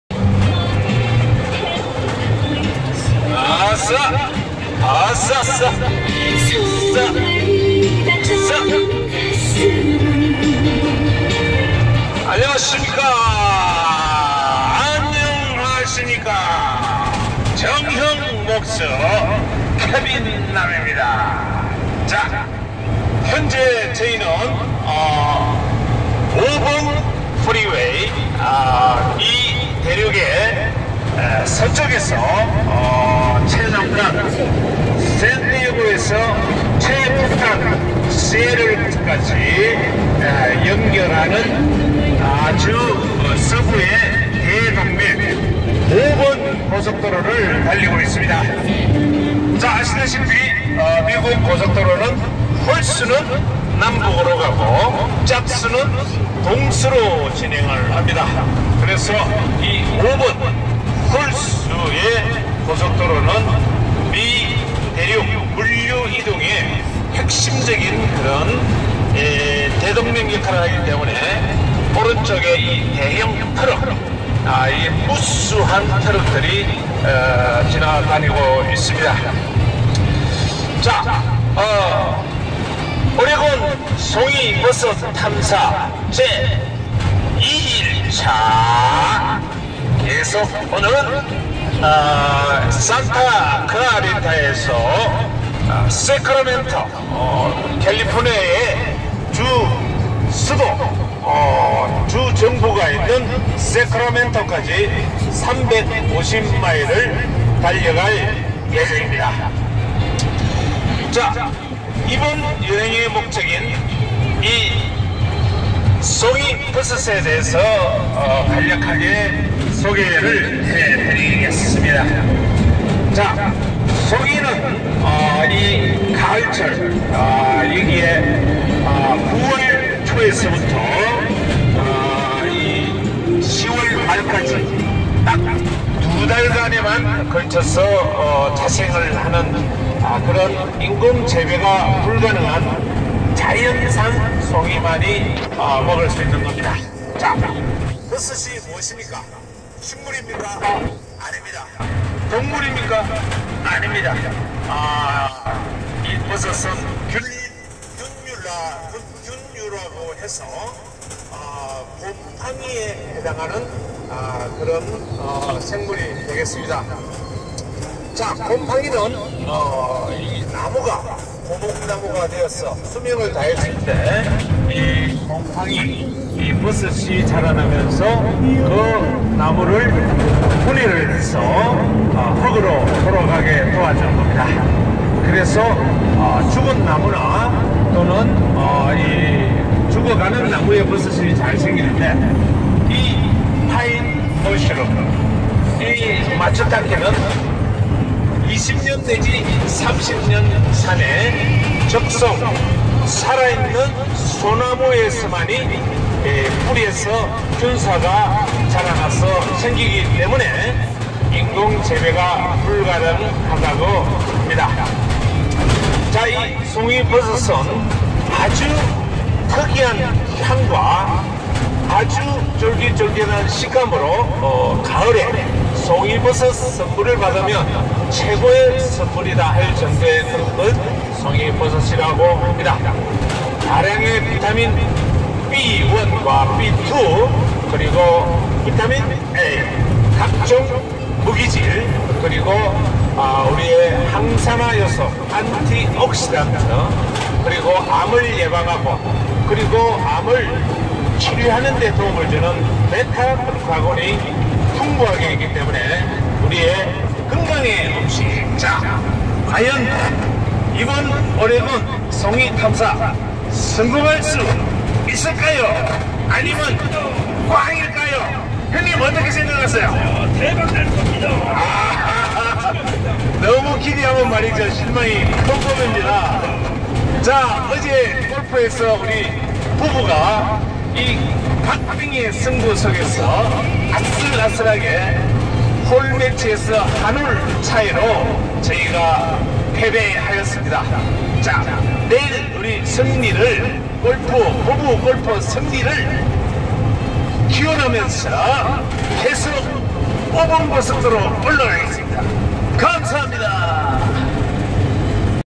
캠핑카와 함께하는 오레곤 송이버섯 탐사
오레곤 송이버섯탐사를 위한 제 2일차, 세크라멘토까지.